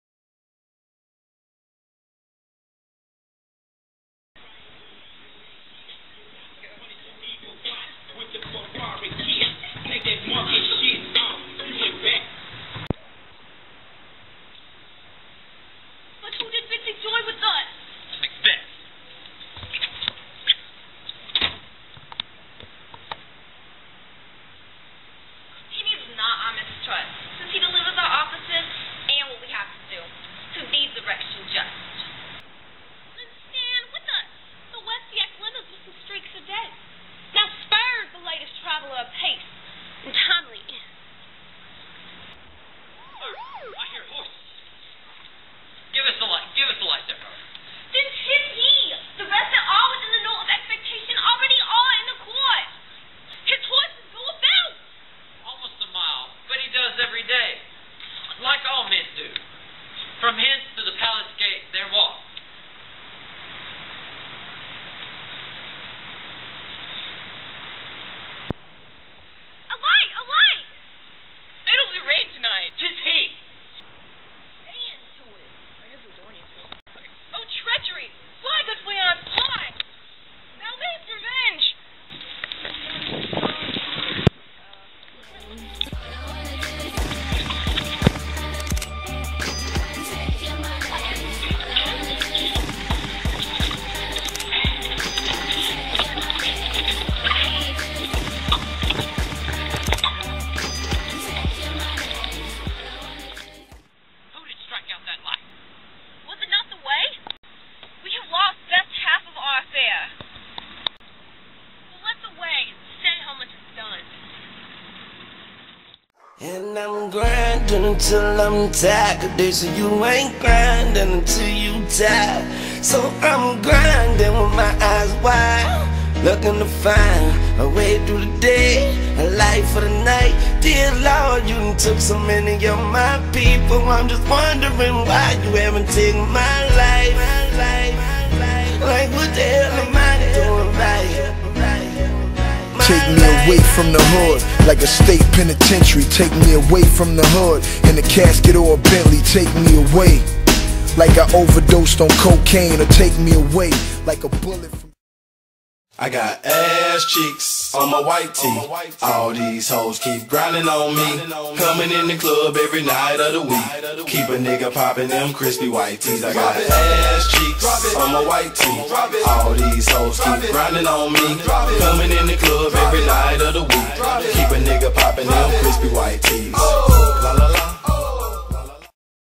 Gangster Macbeth
This is our Extra Credit project for English III Honors. Though a few lines are missing (and the camera/sound quality is horrible), I still think it's pretty funny.